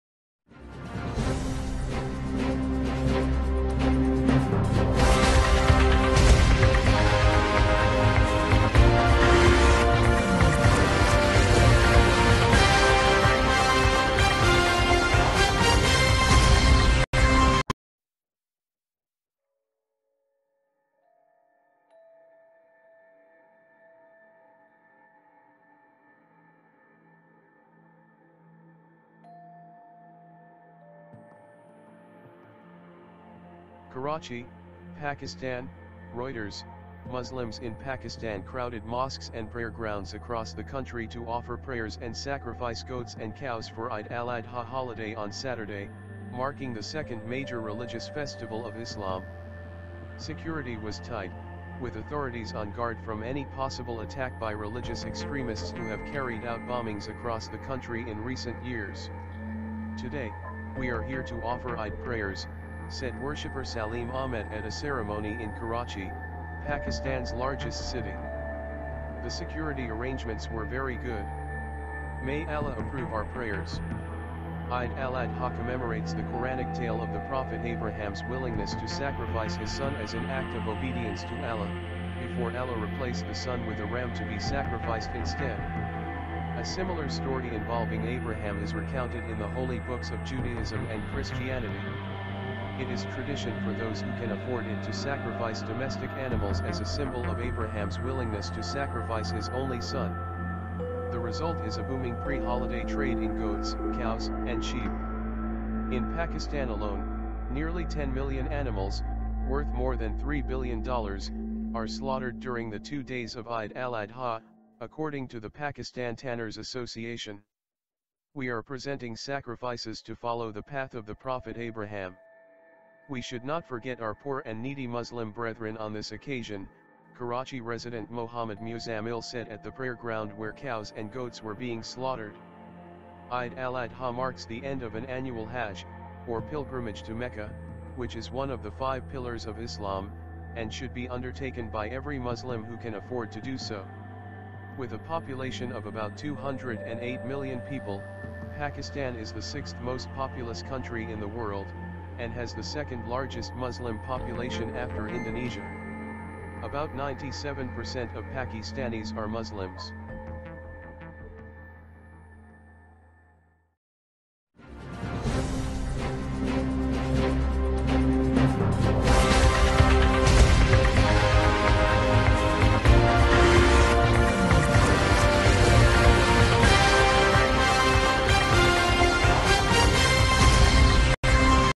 With prayer, sacrifices, Pakistani Muslims celebrate Eid al Adha
sacrifices, Pakistani Muslims celebrate Eid al Adha

KARACHI, Pakistan (Reuters) - Muslims in Pakistan crowded mosques and prayer grounds across the country to offer prayers and sacrifice goats and cows for Eid al-Adha holiday on Saturday, marking the second major religious festival of Islam.

Security was tight, with authorities on guard from any possible attack by religious extremists who have carried out bombings across the country in recent years.

“Today, we are here to offer Eid prayers,” said worshipper Saleem Ahmed at a ceremony in Karachi, Pakistan’s largest city. “The security arrangements were very good. May Allah approve our prayers.”

Eid al-Adha commemorates the Koranic tale of the Prophet Abraham’s willingness to sacrifice his son as an act of obedience to Allah, before Allah replaced the son with a ram to be sacrificed instead. A similar story involving Abraham is recounted in the holy books of Judaism and Christianity.

It is tradition for those who can afford it to sacrifice domestic animals as a symbol of Abraham’s willingness to sacrifice his only son.

The result is a booming pre-holiday trade in goats, cows and sheep. In Pakistan alone, nearly 10 million animals, worth more than $3 billion, are slaughtered during the two days of Eid al-Adha, according to the Pakistan Tanners’ Association.

“We are presenting sacrifices to follow the path of the prophet Abraham. We should not forget our poor and needy Muslim brethren on this occasion,” Karachi resident Mohamad Muzammil said at the prayer ground where cows and goats were being slaughtered.

Eid al-Adha marks the end of an annual Hajj, or pilgrimage to Mecca, which is one of the five pillars of Islam, and should be undertaken by every Muslim who can afford to do so.

With a population of about 208 million people, Pakistan is the sixth most-populous country in the world, and has the second largest Muslim population after Indonesia. About 97 percent of Pakistanis are Muslims.
► Google + :
 ► Tweet :